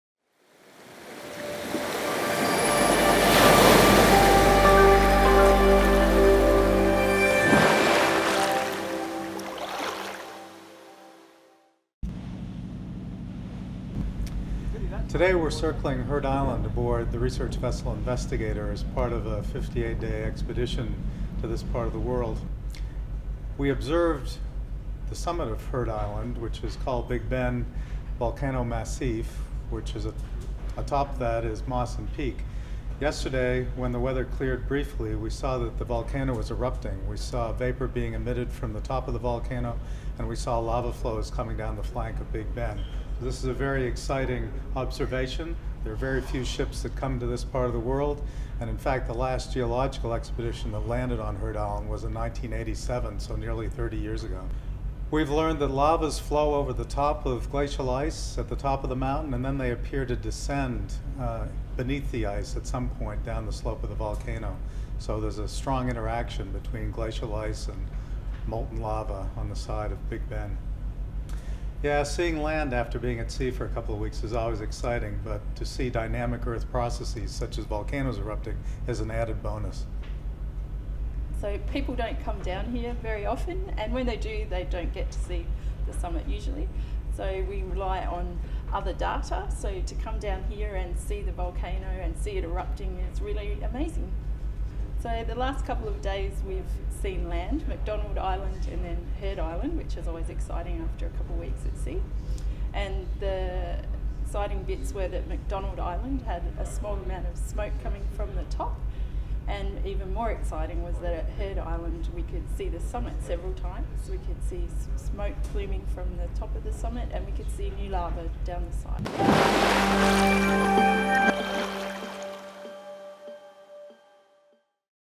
Big Ben Erupts — One of Australia's only two erupting volcanoes goes off
While circumnavigating Heard Island on a research program, the crew of RV Investigator observed and captured rare footage of one of Australia's volcanoes erupting.